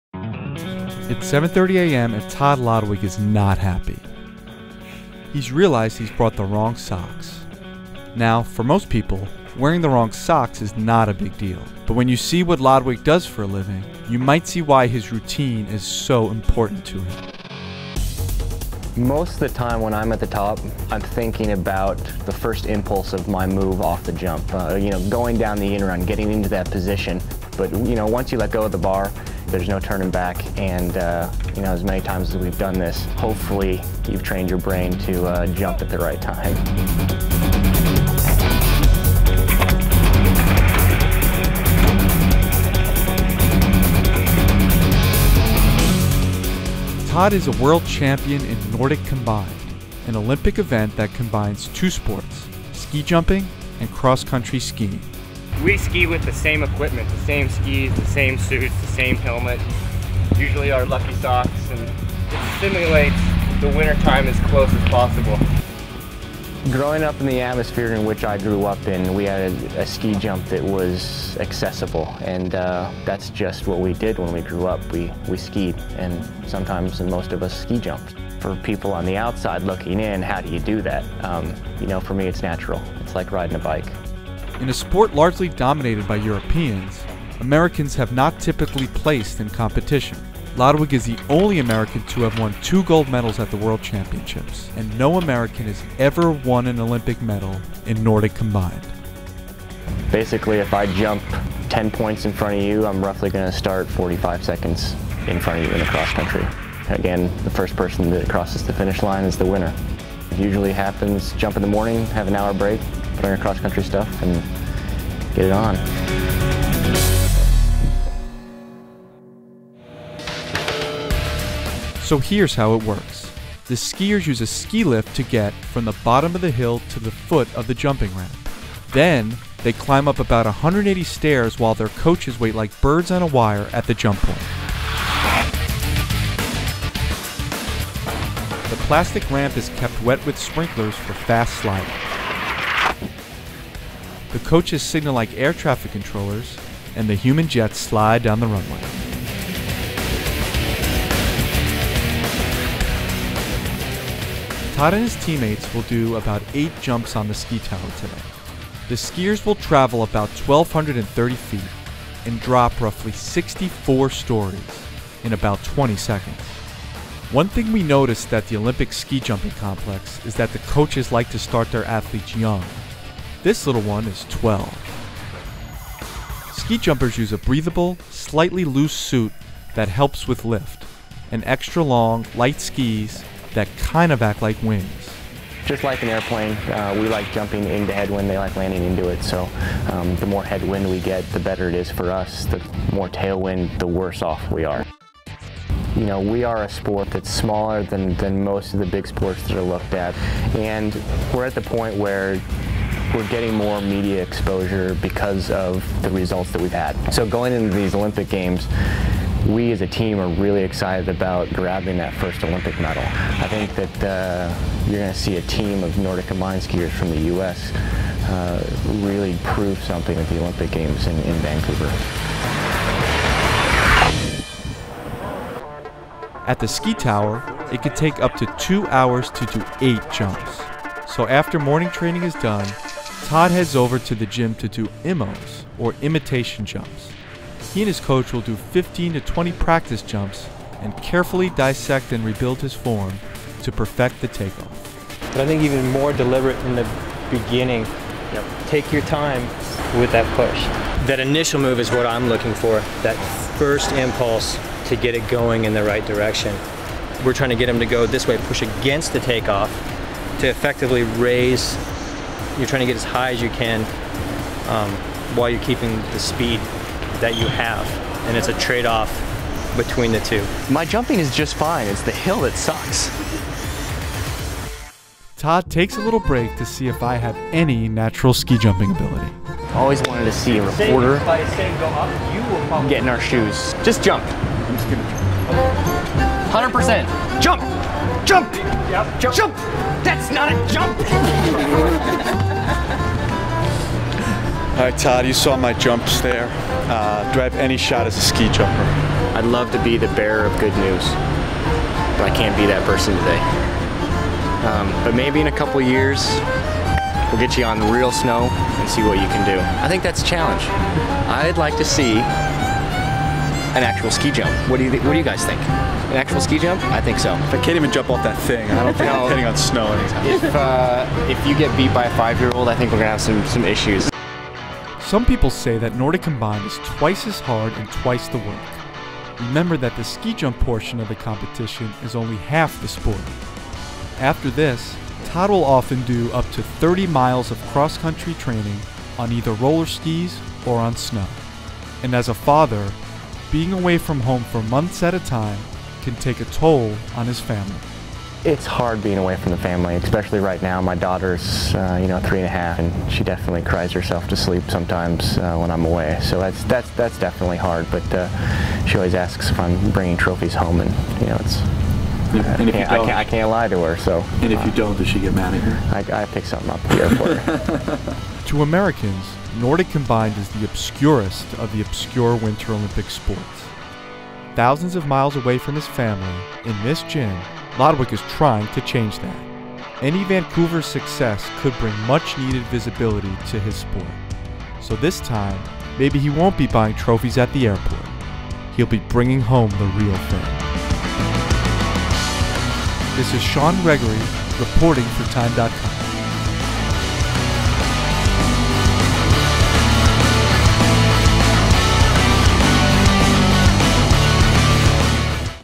Ski Jumping | How They Train | TIME
TIME follows Nordic-combined athlete and Olympic silver medalist Todd Lodwick up a very tall ski tower in Lake Placid, New York, to see how he's training for the winter Olympics in Vancouver.